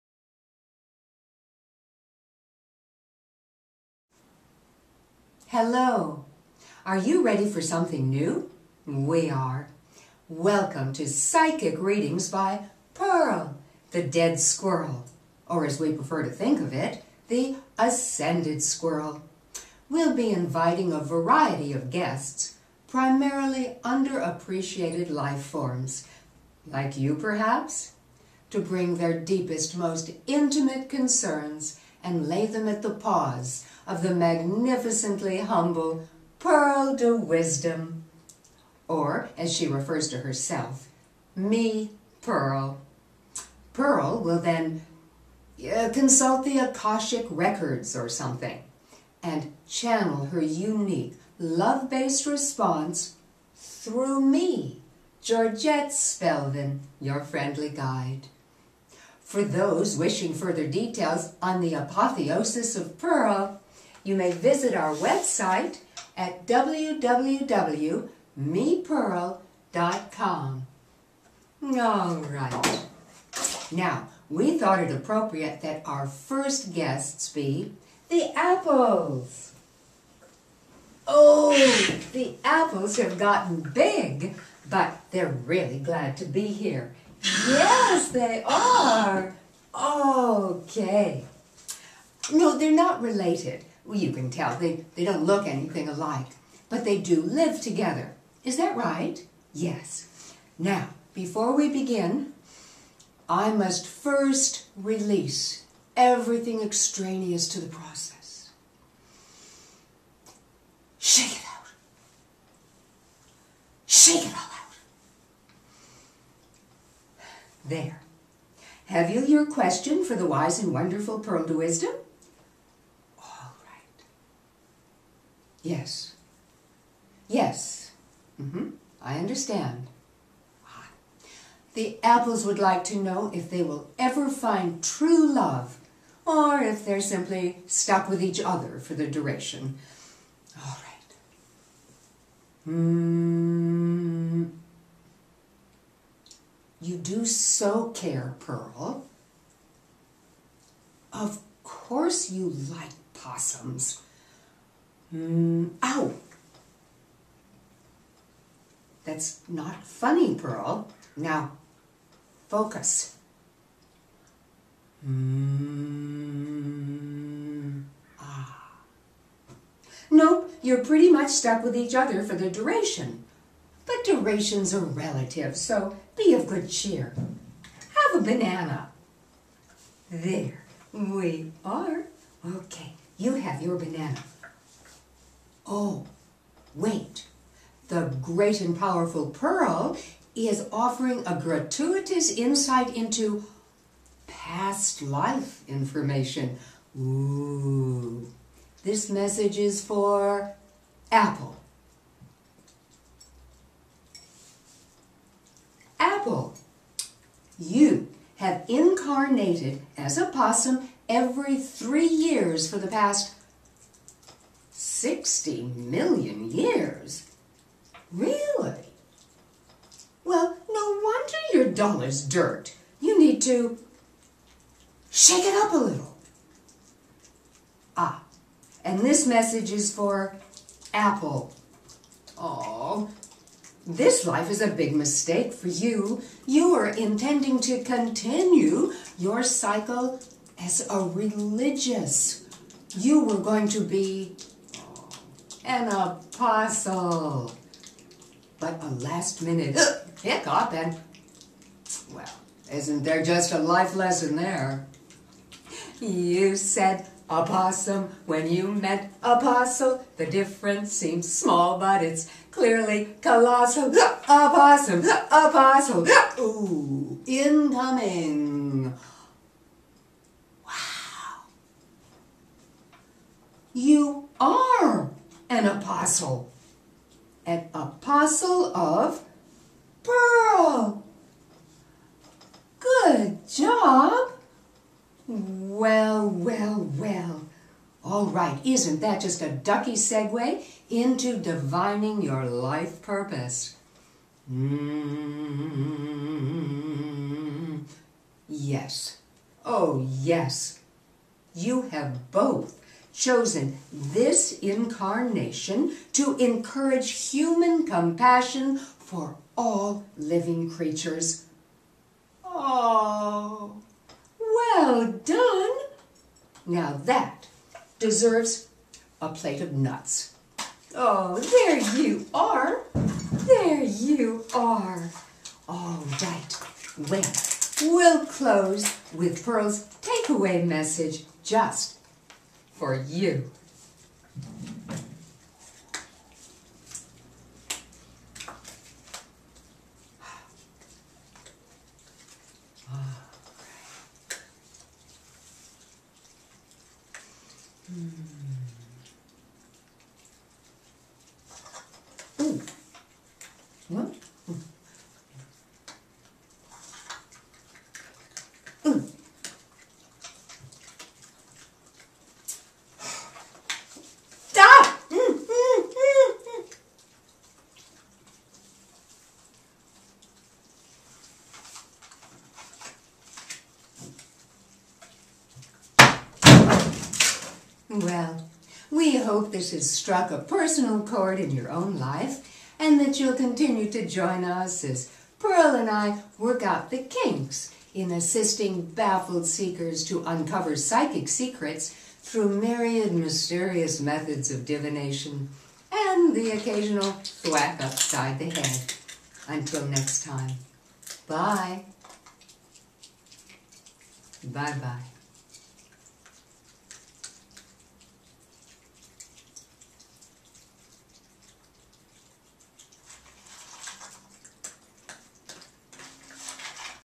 Psychic Readings by Pearl
Psychic life (past and present) readings channeled from an ascended master. This premier episode features the Apples, the popular possum pair seen in the previous series, Proper Opossum Care.